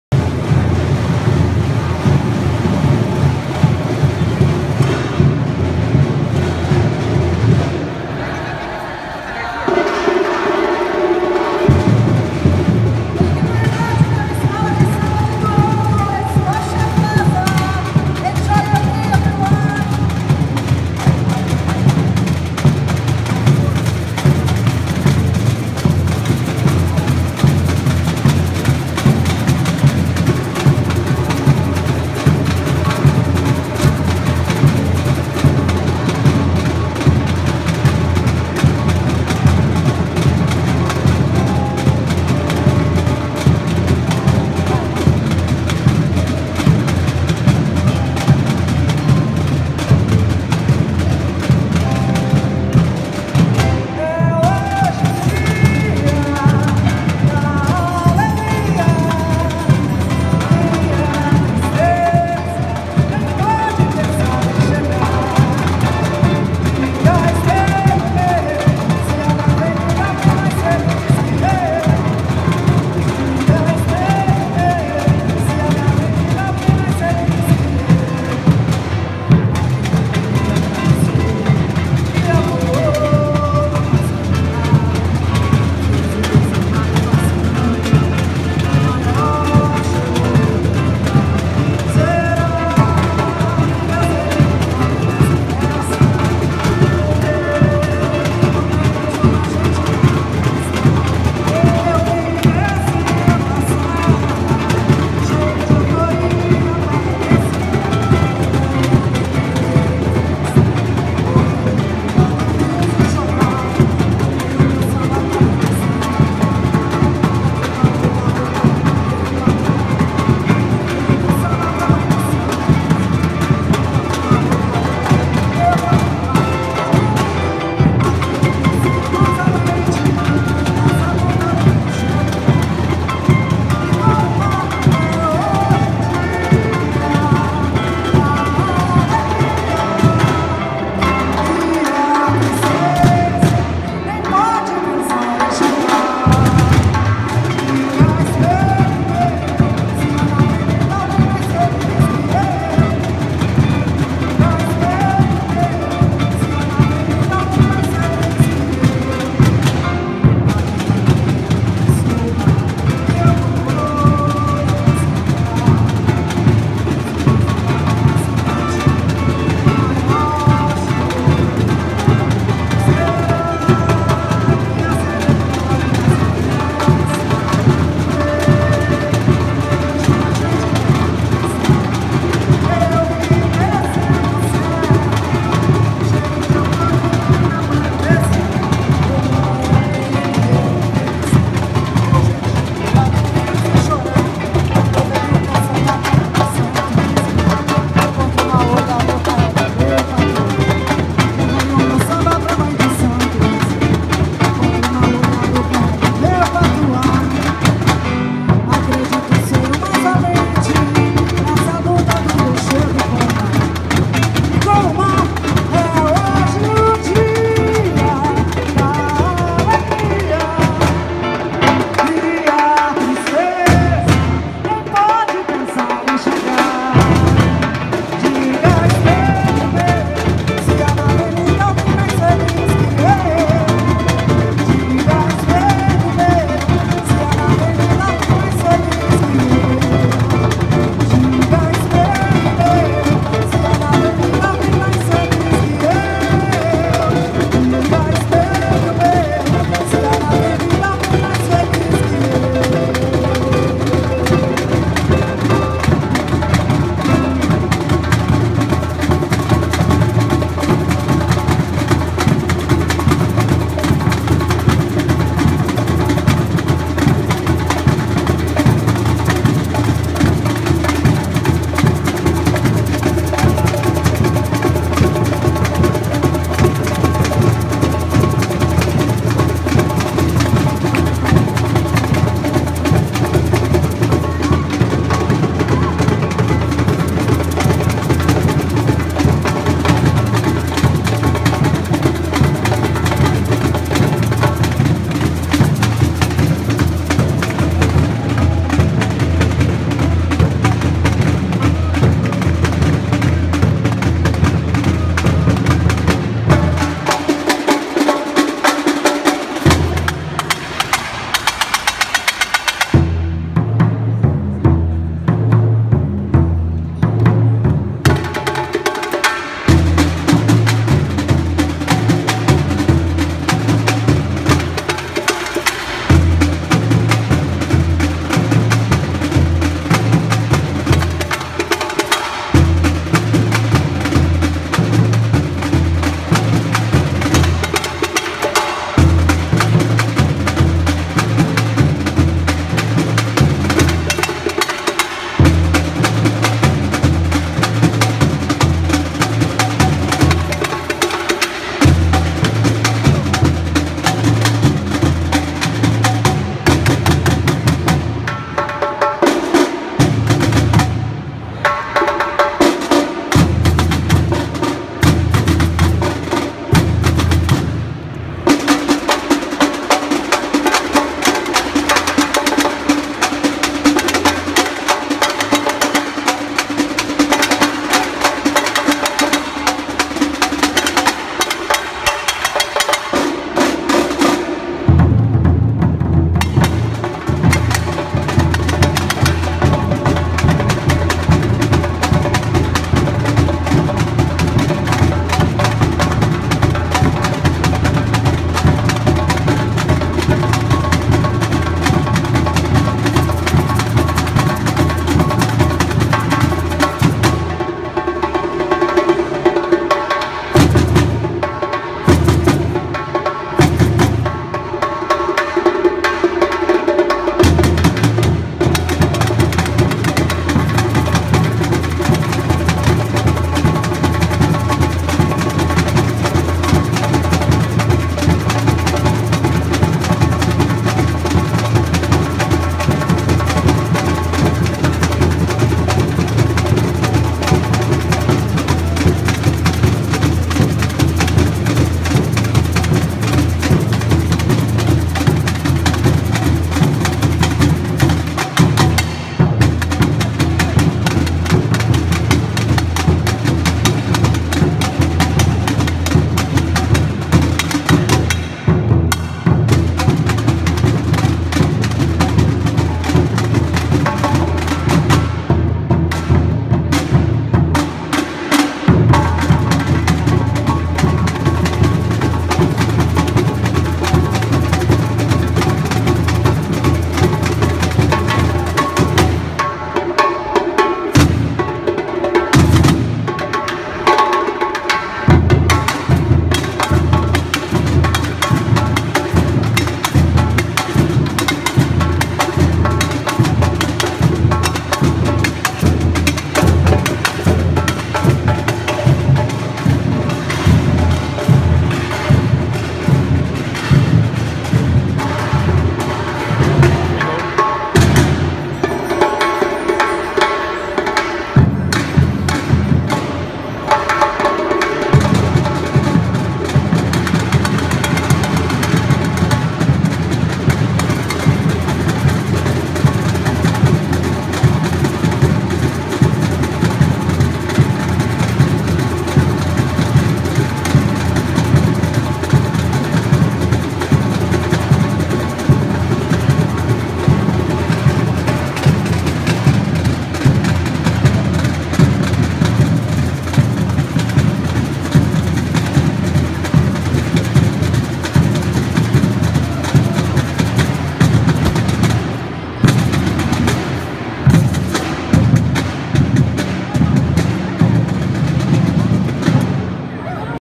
Toronto June 9, 2017 Bateria Drumming Brazilian Samba Dance Scotia Plaza
"A Fantastica Bateria, Acho Melhor!" Brazilian Samba Dance Singing at Scotia Plaza 40 King Street West Toronto Friday June 9, 2017 just after 1pm.